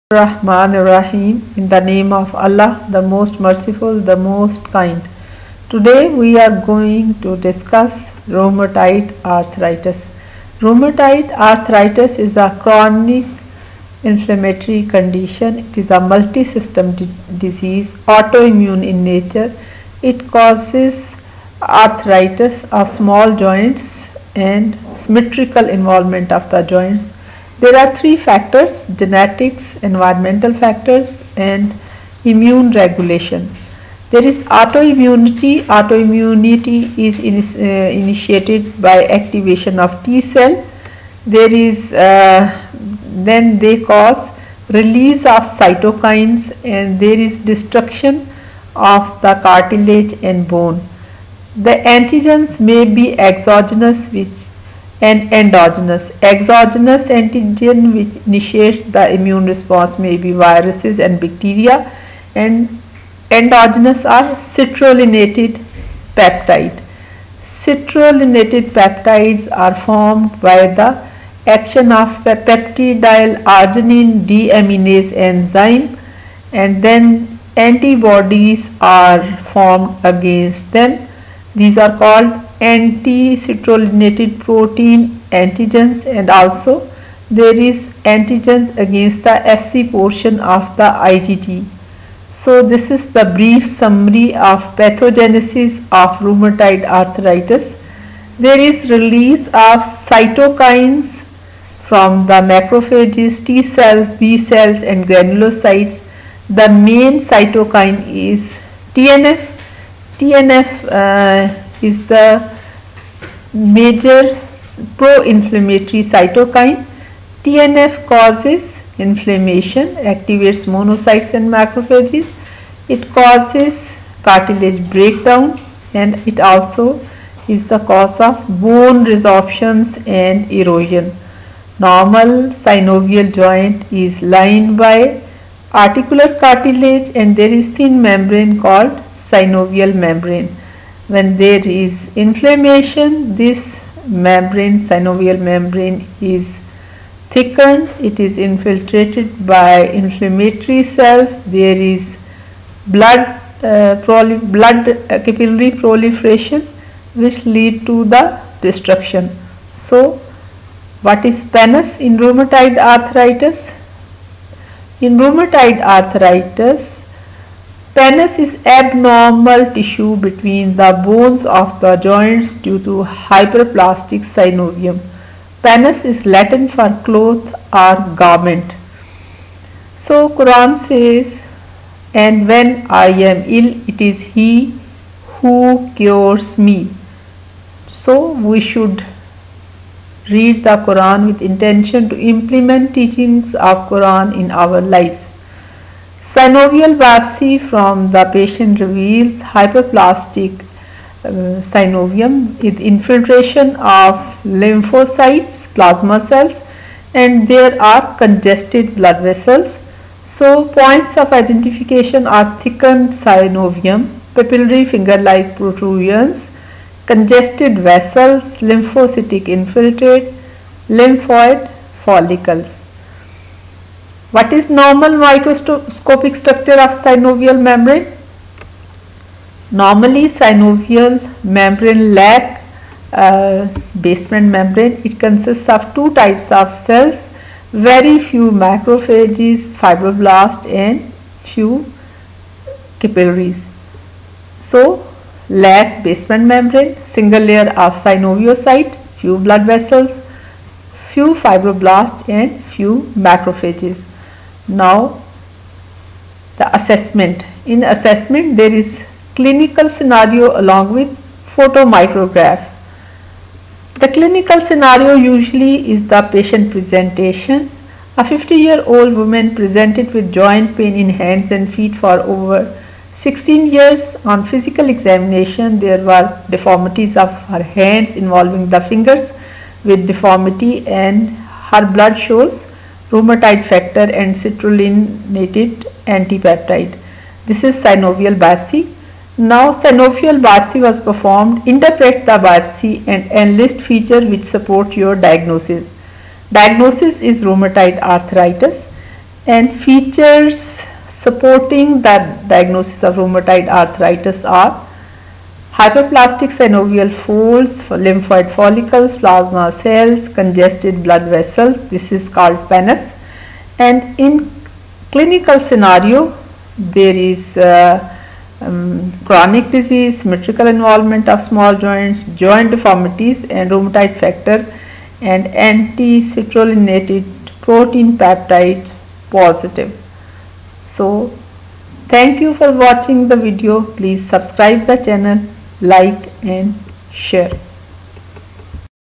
Rheumatoid Arthritis skill lab
Synovial biopsy interpretation in RA for medical students.